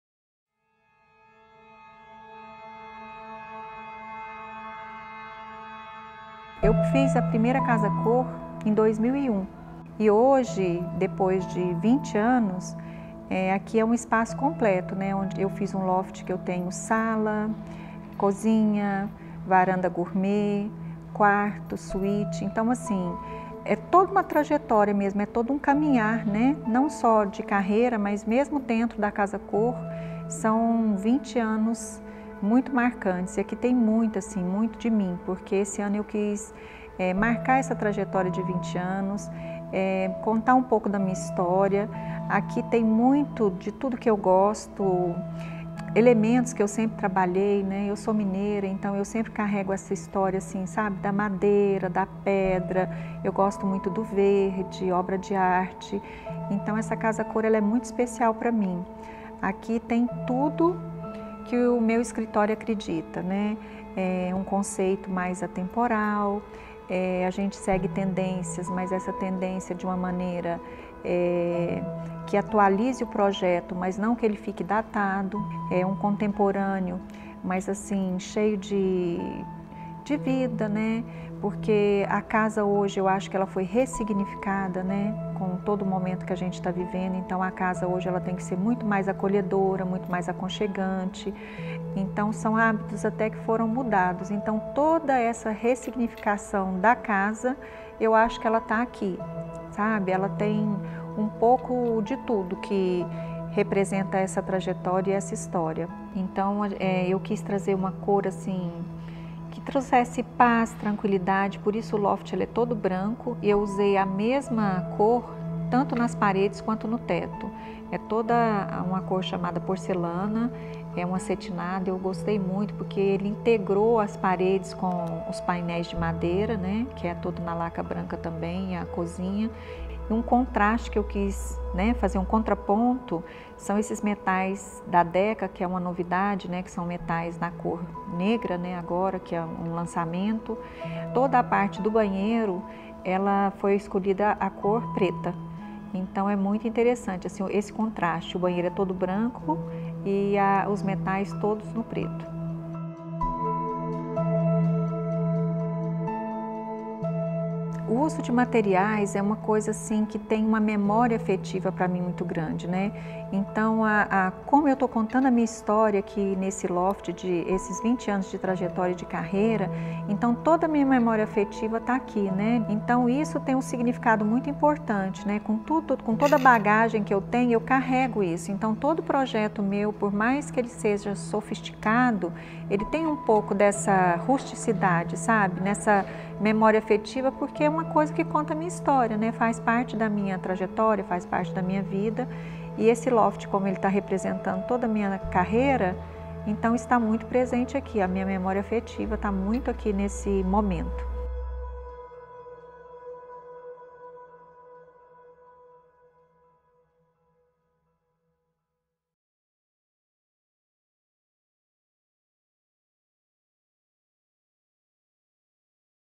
Materiais naturais e tons neutros dão elegância a este loft
Veterana da CASACOR Goiás, a designer de interiores Doriselma Mariotto retorna à mostra em sua 18ª participação com o Loft Trajetória 20 de 235 m², um espaço atemporal e contemporâneo inspirado em seus 20 anos de carreira.

Destinado ao público que aprecia morar bem, com elegância e conforto, o ambiente não segue tendências e se destaca pelos materiais naturais como pedras e madeira.